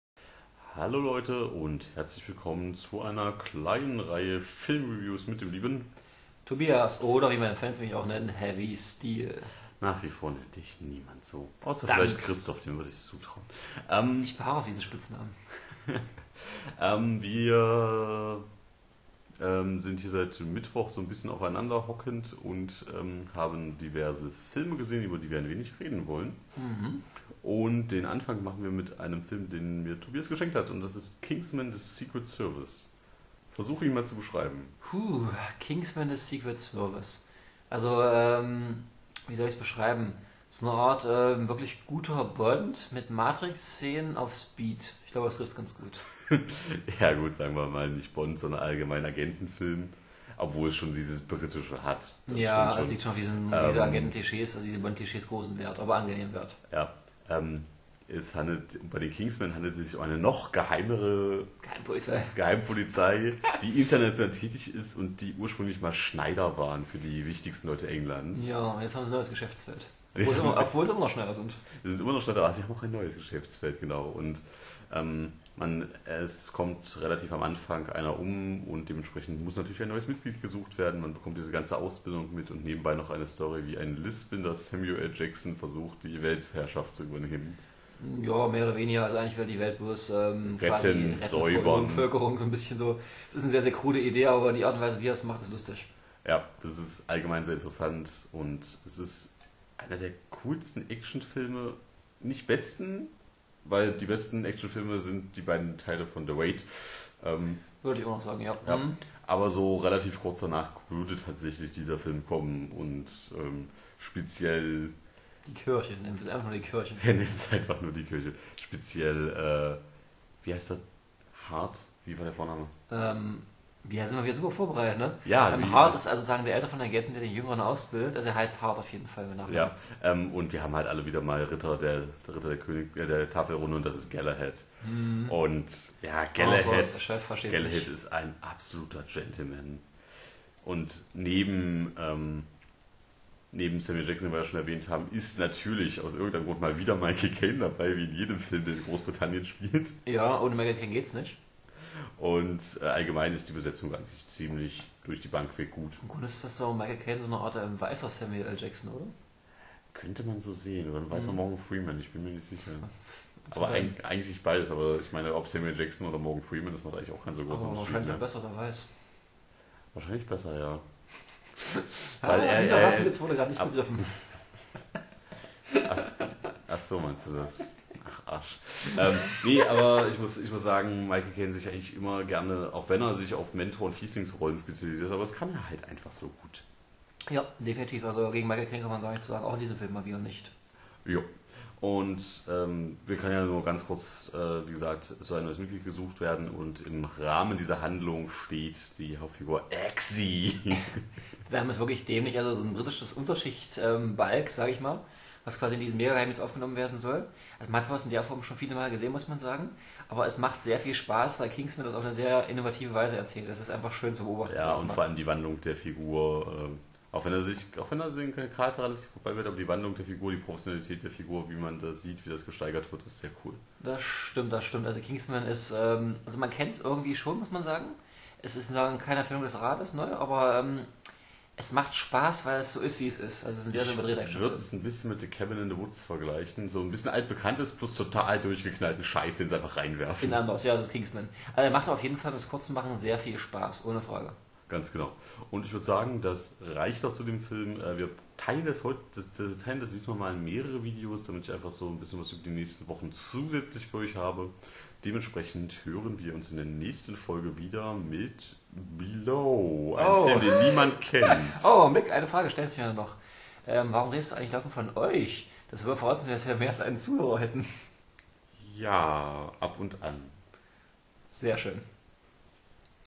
Kingsman Filmbesprechung (mit Tobias)
Action, flotte Sprüche und Schneider. Wie das alles zusammenpasst versuchen wir hier zu besprechen.